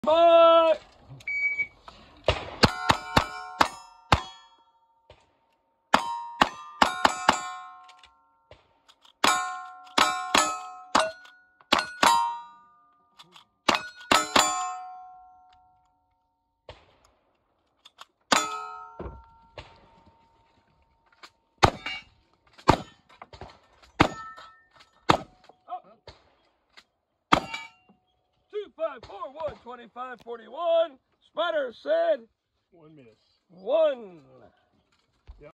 Stage 6 - the OTJ - Raiders - 12/5/20
Stage 6 had it all. Another AD with the rifle in which I hit a pistol target causing a miss, a jacked round, and my first make up of the day on my very last shotgun knockdown. The miss caused me to slip from 4th Overall to 6th. It was a character builder no doubt.

Top 5:
1) Arcadia Outlaw
2) Santa Fe River Stan
3) JP Law
4) Bucky Buckskin
5) Dominator

Top Lady:
Hawkeye Gin

Clean Shooters: Arcadia Outlaw, JP Law